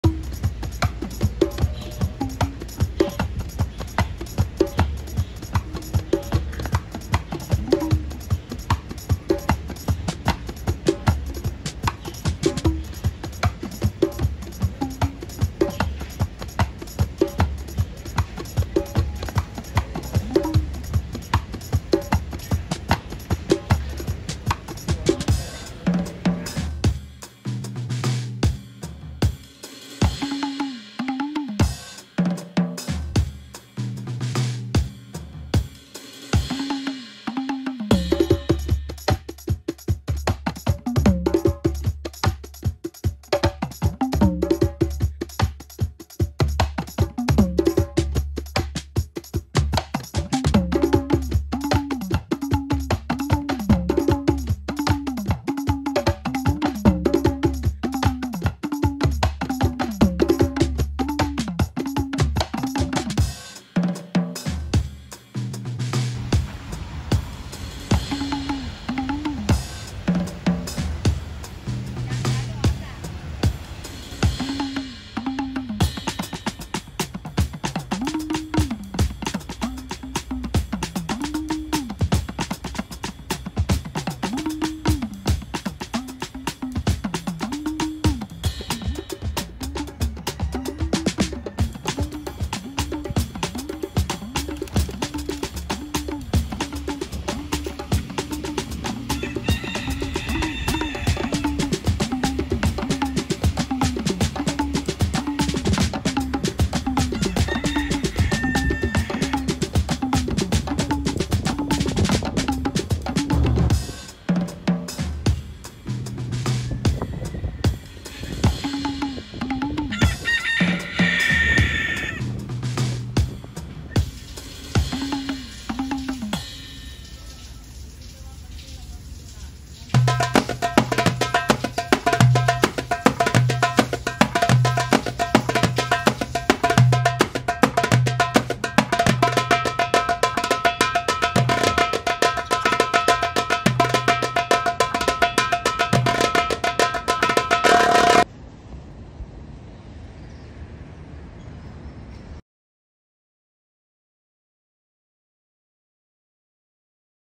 Roosters, Pigeons and Grackles
This is a video along some recent pictures of roosters, grackles (changos in Puerto Rico) and pigeons, which I took and recorded on different areas of the metro area of the island. You can also check the recent post on the blog section.